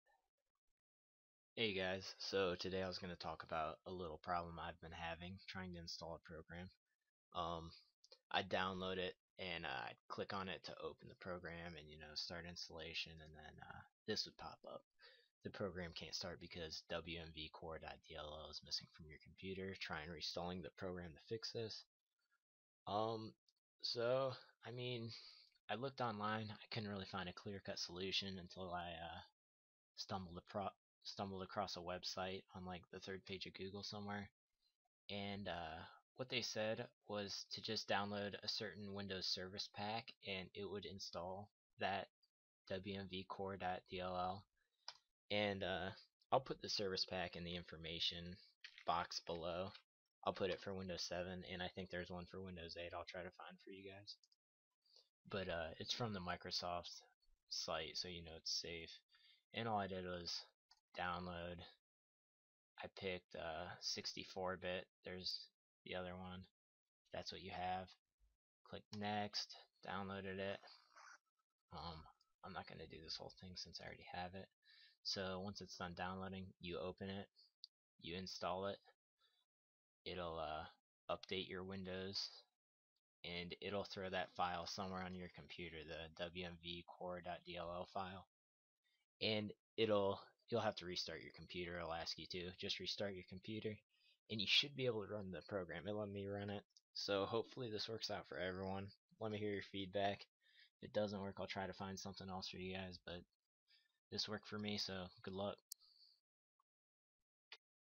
[FIX] WMVCore.DLL is missing from your computer
I recently got this error has stated above, when I was trying to open a program for installation. I'm not sure the other ways this error will pop up on computers, but hopefully this quick solution will help you guys out. My computer is pretty new so I don't think I ever messed with the WMVCore.dll file on mine so some may just come without it. Good luck!

*Errors that may pop up from the result of missing WMVCore.DLL:

The program can’t start because WMVCORE.DLL is missing from your computer. Try reinstalling the program to fix this problem.

Wmvcore.dll Not Found

Cannot find [PATH]\wmvcore.dll

The file wmvcore.dll is missing

 A required component is missing: wmvcore.dll.